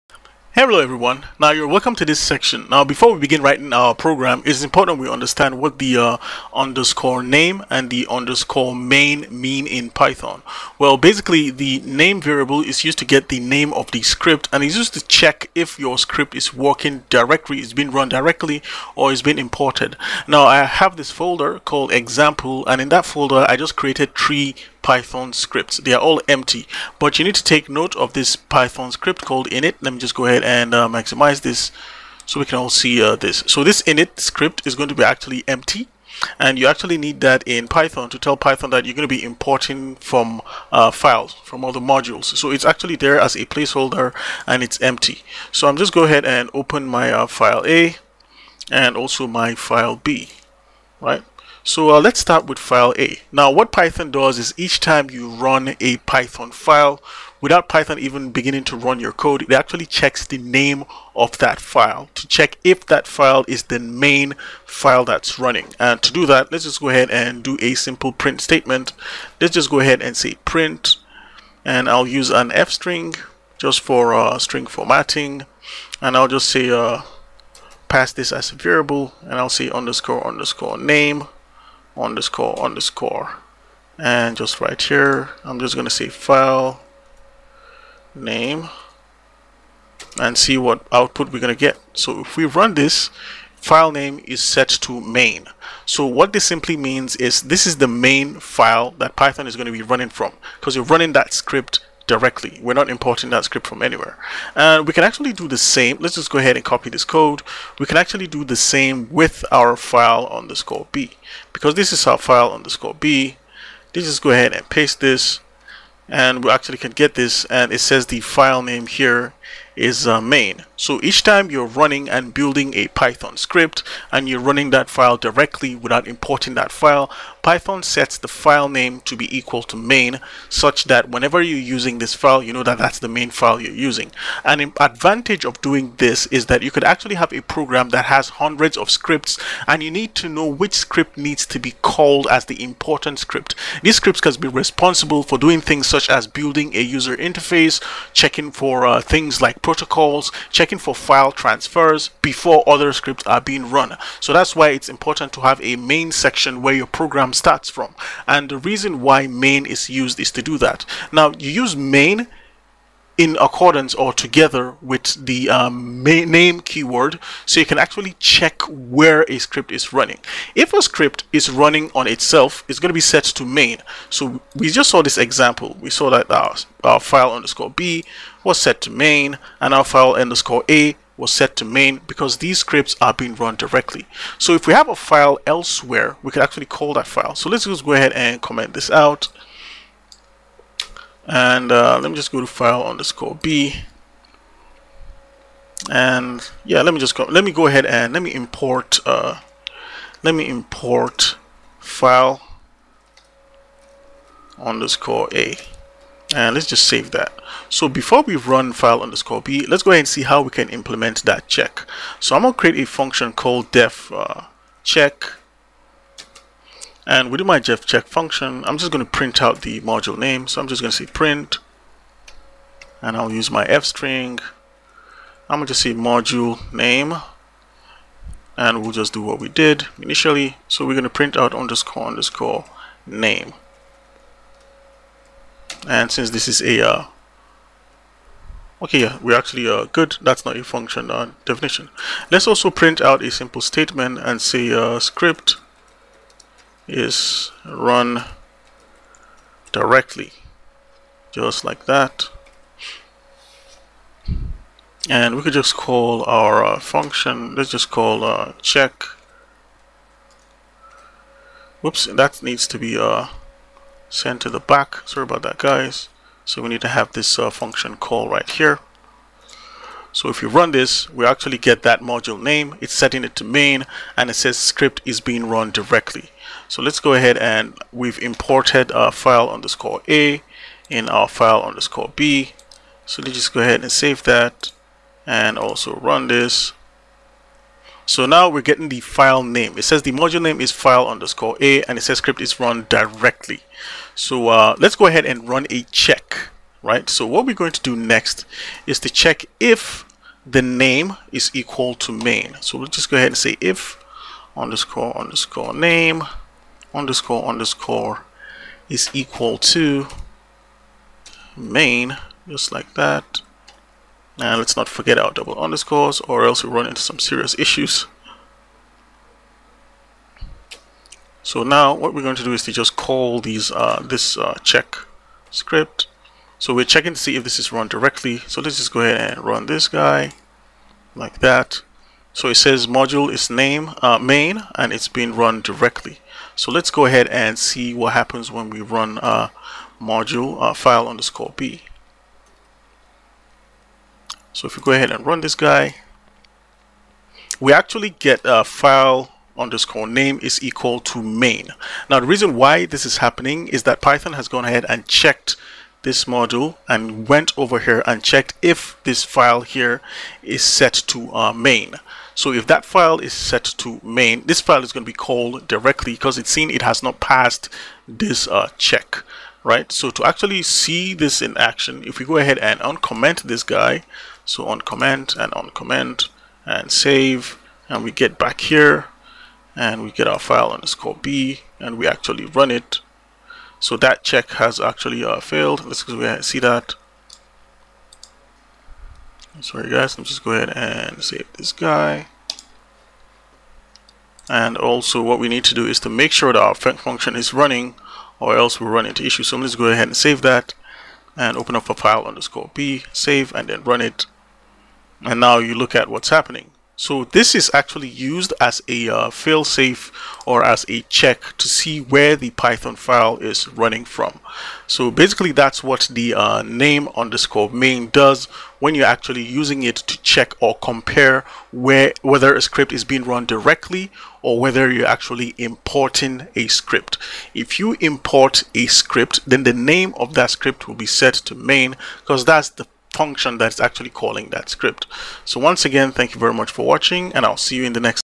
What are Pythons __name__ and __main__ methods?
this video aims to demystify and teach viewers the meaning of the name and main dunder methods.
wanna learn more?